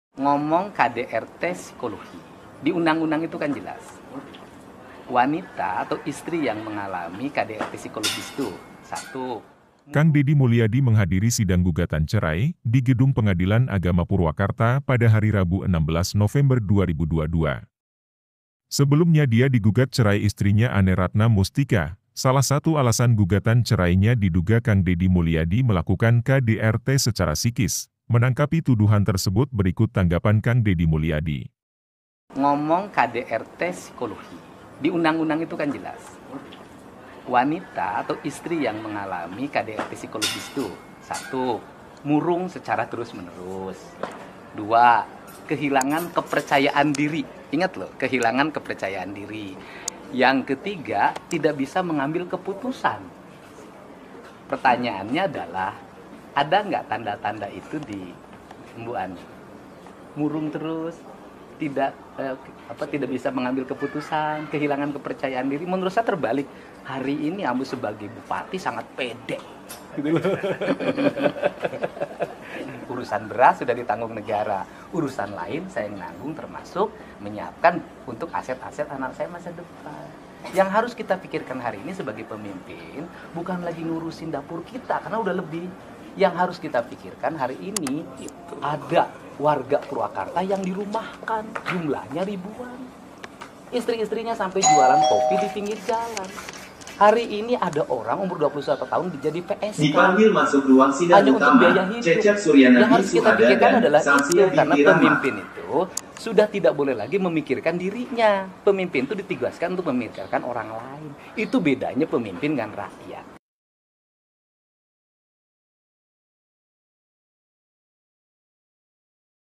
TANGGAPAN KANG DEDI MULYADI ATAS TUDUHAN KDRT SIKOLOGIS
Kang dedi mulyadi menghadiri sidang gugatan cerai, di gedung pengadilan agama purwakarta pada hari rabu 16 november 2022.sebelum nya dia di gugat cerai istrinya ane ratna mustika,salahsatu alasan gugatan cerai nya diduga kang dedi mulyadi melakukan K D R T secara sikis, menangkapi tuduhan tersebut berikut tanggapan kang dedi mulyadi.